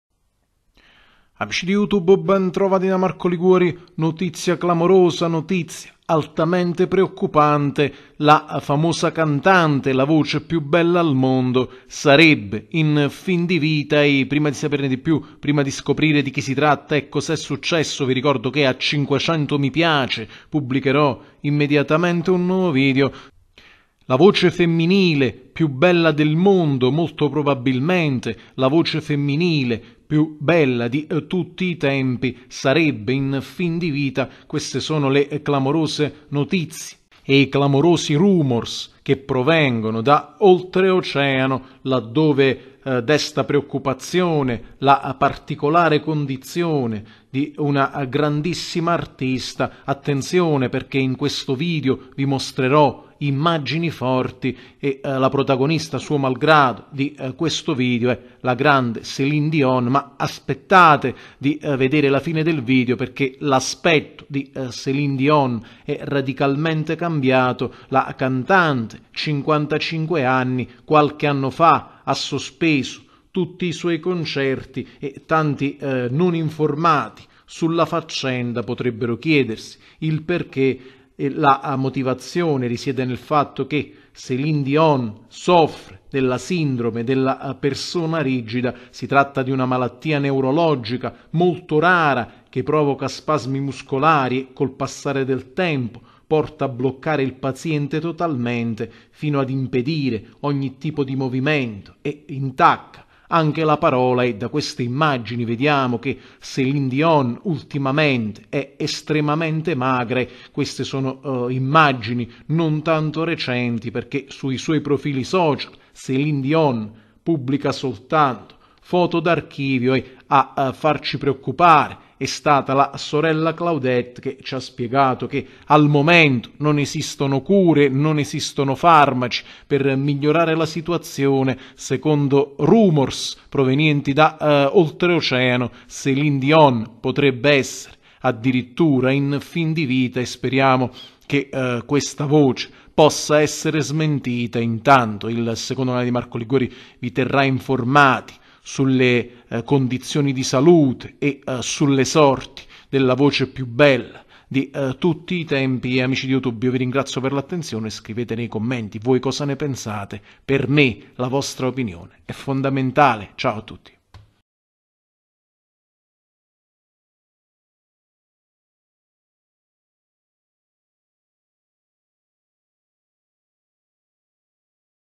E' IN FIN DI VITA LA CANTANTE CON LA VOCE PIU' BELLA AL MONDO! NOTIZIA ARRIVATA NELLA NOTTE...
Notizia shock dall'America su Celine Dion.